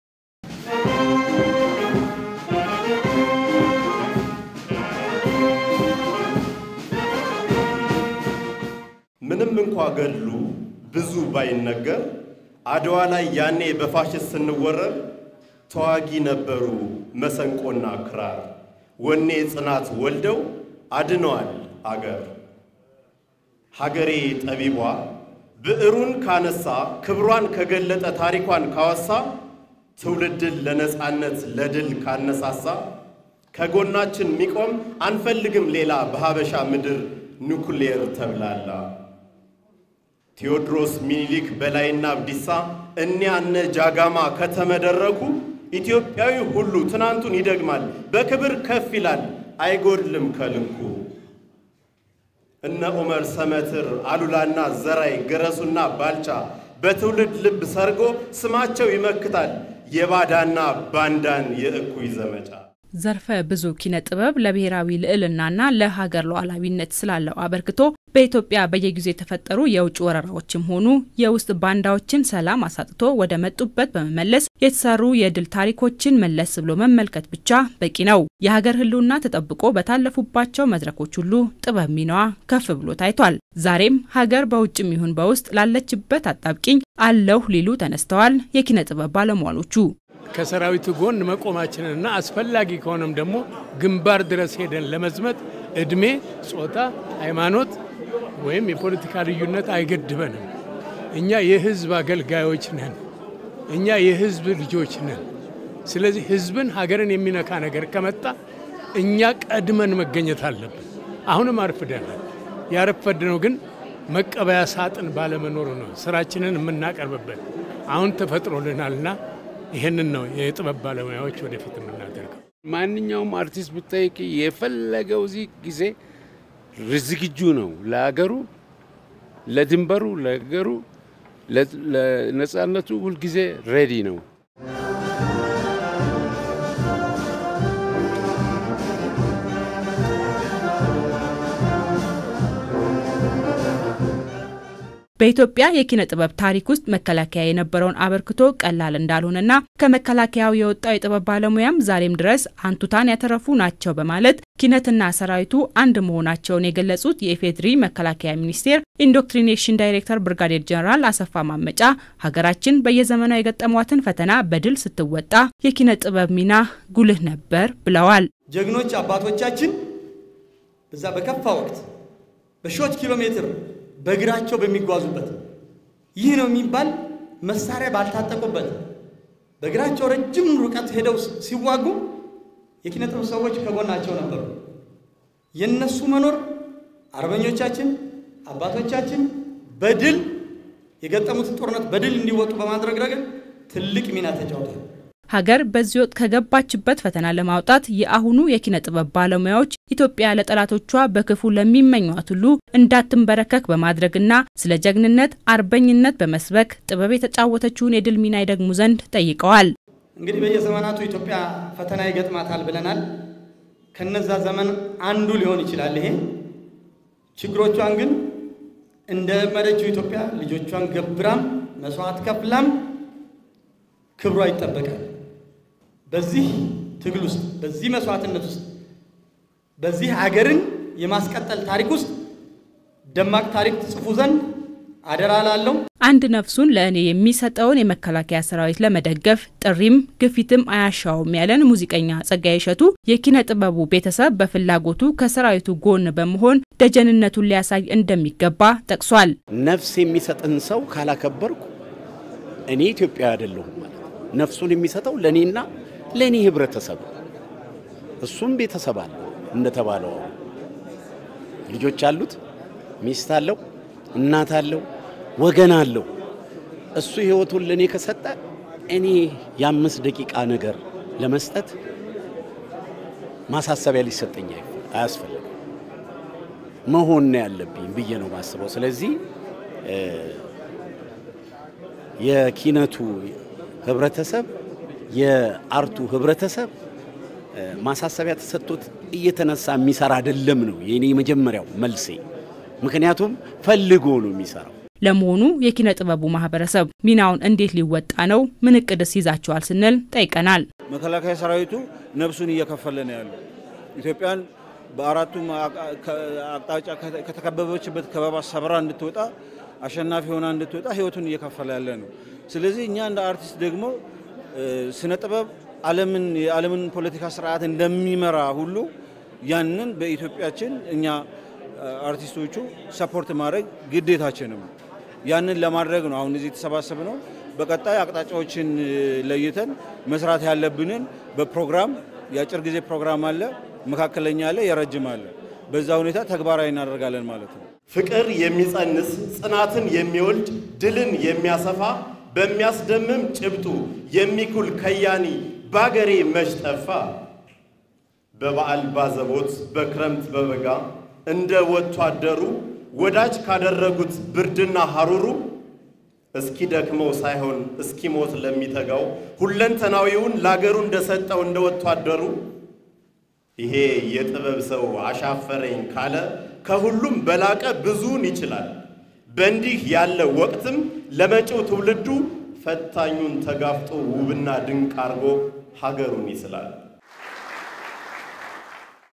ለኢትዮጵያ ማሸነፍ የኪነ-ጥበብ ሚና
ኢትዮጵያ በየዘመኗ የገጠሟትን ችግሮች በአሸናፊነት ስትወጣ ኪነ-ጥበብ ሚናዋ የላቀ መሆኑ እና ይህም የእስካሁን እውነታ መሆኑ ተገልጿል ዛሬ በነበረ አንድ ሃገራዊ መድረክ ላይ።

የኪነጥበብ ባለሙያዎች ኢትዮጵያ በዚህ ወቅት ከገጠማት ፈተና ለመወጣት በሚደረገው ርብርብ የድርሻቸውን እንዲያበረክቱ ተጠይቋል።

የኪነጥበብ ቤተሰቡም ለመከላከያ ሰራዊት ደጀንነቱን ለማሳየት ቁርጠኛ መሆኑን ገልጿል።